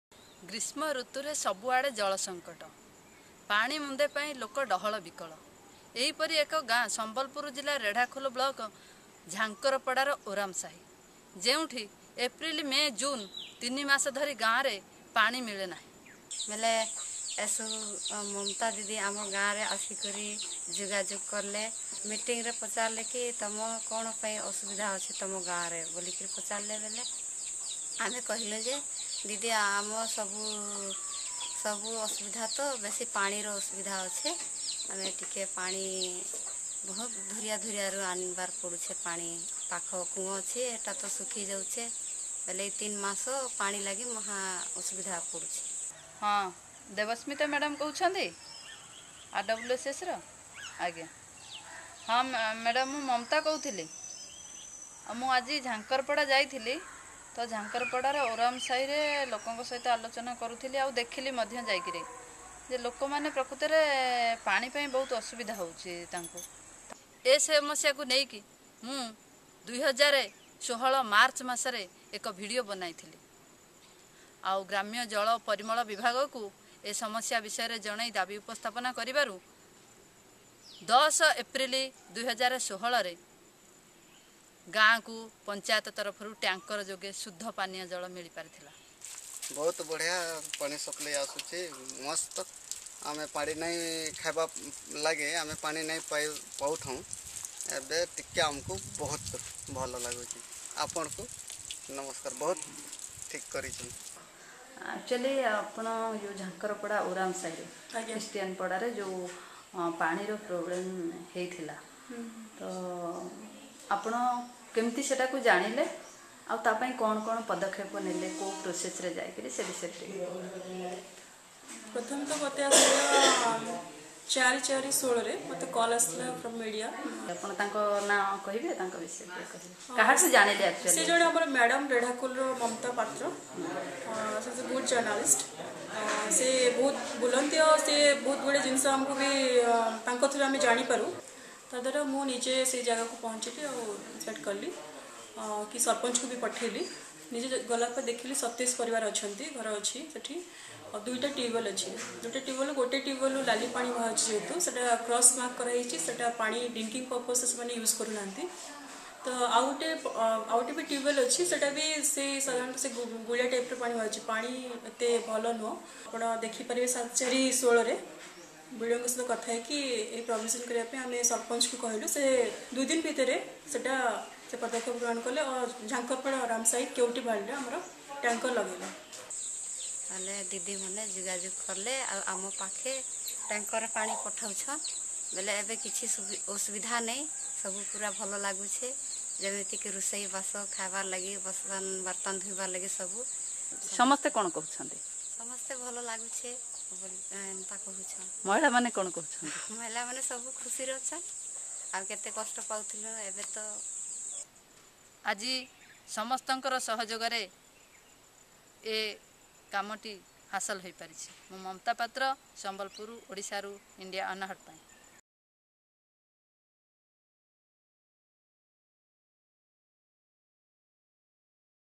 Residents walk 1.5 Km to fetch water
Mamta Patra reports from Sambalpur, Odisha for Video Volunteers.

iCountryRide
coutryridei